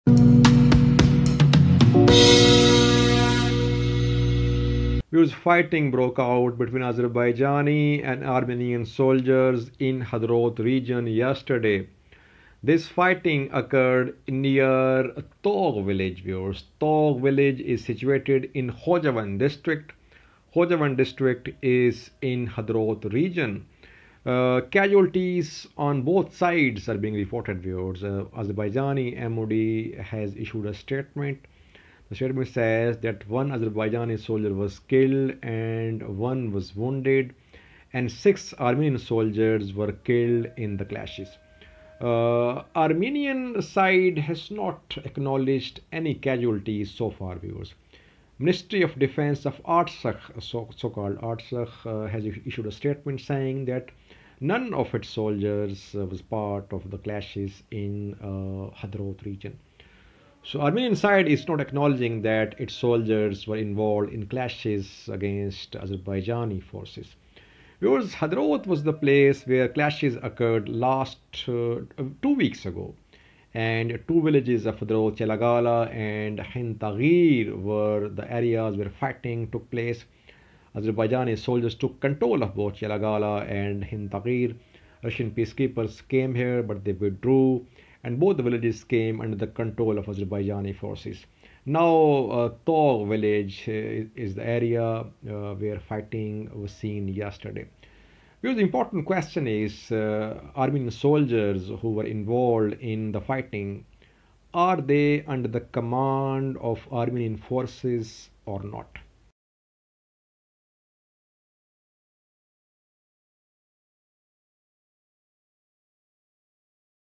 Fresh fighting between Azerbaijan & Armenia near Tug village of Hadrut
Fresh fighting erupted between the forces of Armenia and Azerbaijan near Tug village of Hadrut region. According to Azerbaijan Ministry of Defense, 1 Azeri soldier was killed, 1 was wounded and 6 Armenian soldiers were killed.  Nagorno Karabakh government said its soldiers were not part of clashes in Tug village of Khojavend district. Azerbaijan and Armenia signed a ceasefire deal on November 10 after having fought a 44-day long deadly war.